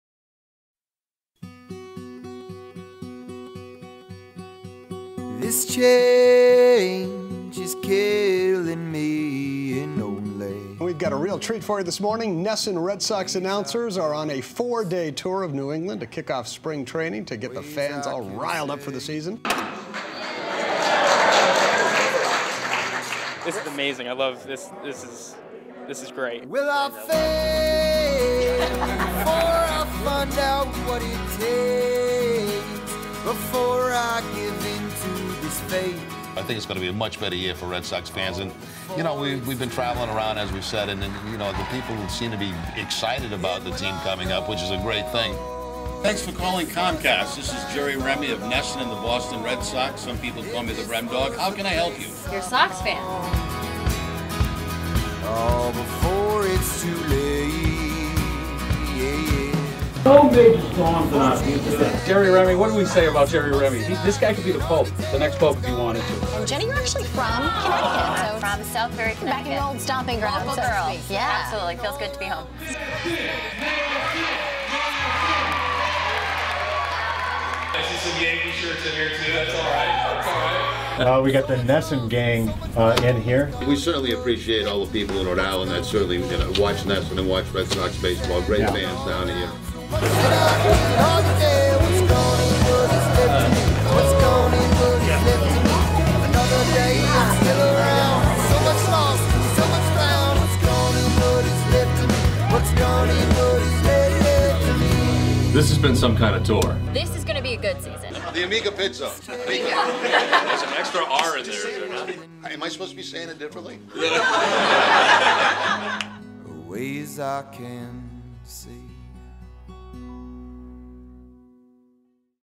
NESN Spring Training Kick Off Tour Highlights
Don Orsillo, Jerry Remy and Jenny Dell traveled to five New England cities in four days to kick off NESN's coverage of Red Sox spring training in 2013. Check out the highlights of the trip in this video.